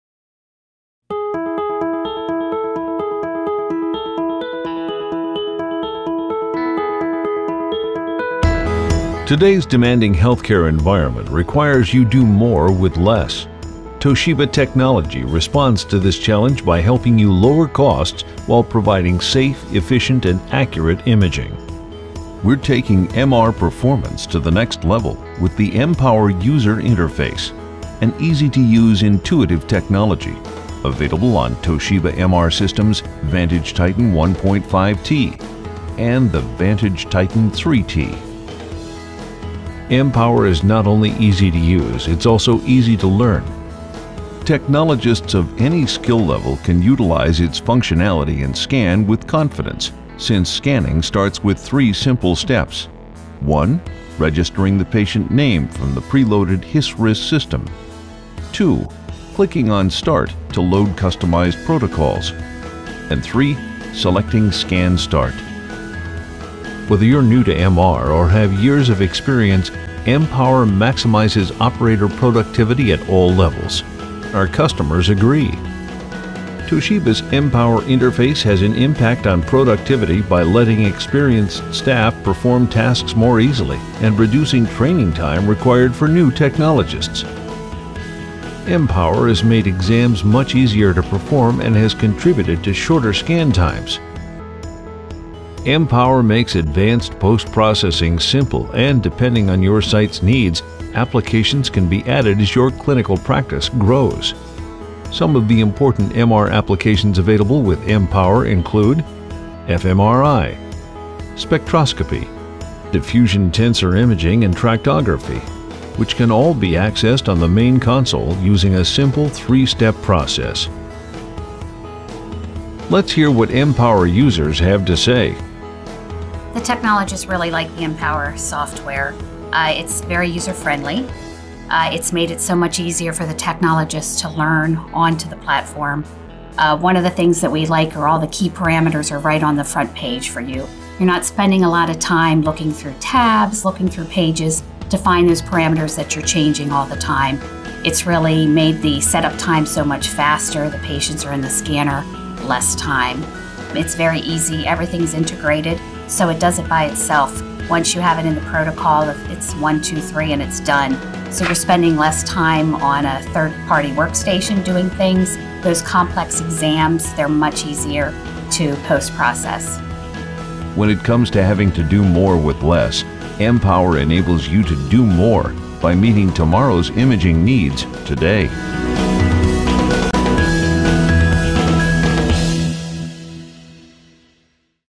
M-Power Overview
M-Power is an intuitive user interface built for all levels of users. It allows an efficient workflow and provides advanced post processing software with ease of use in mind on the main console. Eliminating the need to purchase multiple 3rd party software for your advanced imaging needs.